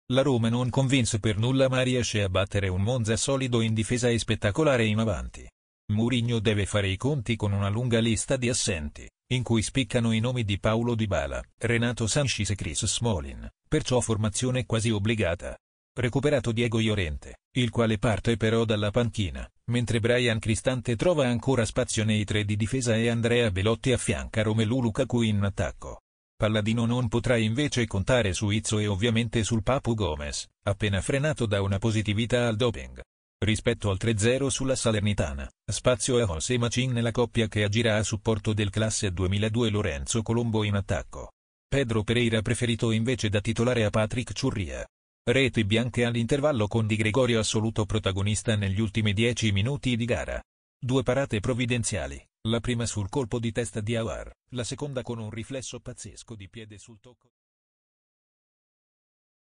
Roma - Monza 1-0 | 9a Giornata seriea 2023/24 (90' elshaarawy )
La Roma non convince per nulla ma riesce a battere un Monza solido in difesa e spettacolare in avanti. Mourinho deve fare i conti con una lunga lista di assenti, in cui spiccano i nomi di Paulo Dybala, Renato Sanches e Chris Smalling, perciò formazione quasi obbligata. Recuperato Diego Llorente, il quale parte però dalla panchina, mentre Bryan Cristante trova ancora spazio nei tre di difesa e Andrea Belotti affianca Romelu Lukaku in attacco. Palladino non potrà invece contare su Izzo e ovviamente sul Papu Gomez, appena  frenato da una positività al doping. Rispetto al 3-0 sulla Salernitana, spazio a José Machin nella coppia che agirà a supporto del classe 2002 Lorenzo Colombo in attacco. Pedro Pereira preferito invece da titolare a Patrick Ciurria. Reti bianche all'intervallo con Di Gregorio assoluto protagonista negli ultimi 10 minuti di gara. Due parate provvidenziali, la prima sul colpo di testa di Aouar, la seconda con un riflesso pazzesco di piede sul tocco da due passi di Belotti. Al netto delle due immense occasioni da gol, la Roma non ha impressionato più di tanto per la qualità di calcio proposta. Monza meglio degli avversari in fase di possesso palla, eppure pochissime verticalizzazioni e praticamente nessun tiro verso la porta di Rui Patricio. Nella seconda frazione, La Roma fa fatica anche con l'uomo in più (Espulso D'Ambrosio) con gli ospiti arroccati in difesa e pochi spazi a disposizione. Nel finale, l'assedio giallorosso, stavolta paga. Sponda di Kristensen sul cross di Zalewski, El Shaarawy prolunga di testa per Azmoun, il cui tiro viene però murato dalla difesa biancorossa. La palla resta lì ed il "Faraone" trafigge Di Gregorio con un destro a centro area che fa impazzire l'Olimpico. I tre punti alla fine sono arrivati, ma con parecchia fatica.